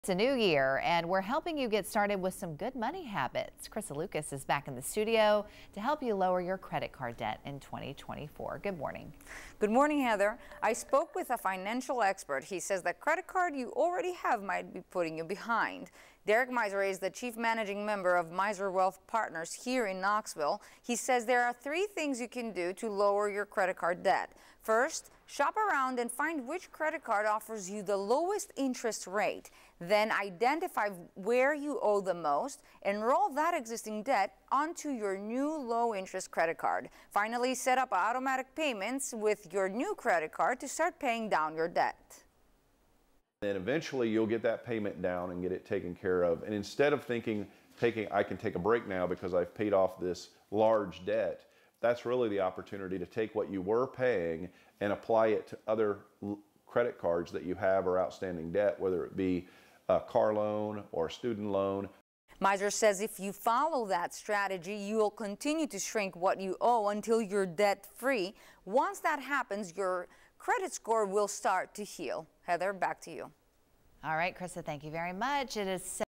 Simple steps to save money, lower debt, kickstart 2024 with good money habits
An East Tennessee financial expert shares tips on how to save money and lower credit card debt.